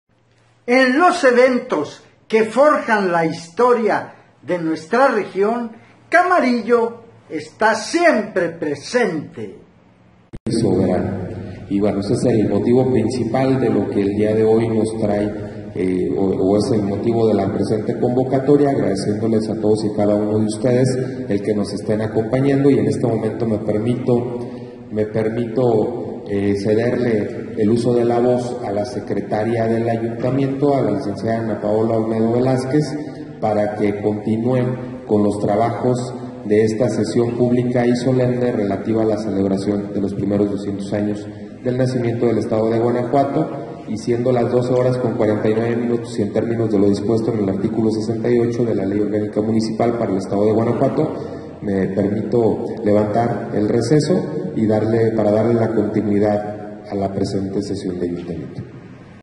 CAMARILLO AL INSTANTE. GOYO MENDOZA preside Sesión de Ayuntamiento
Festejando 200 años de que Guanajuato es Entidad Federativa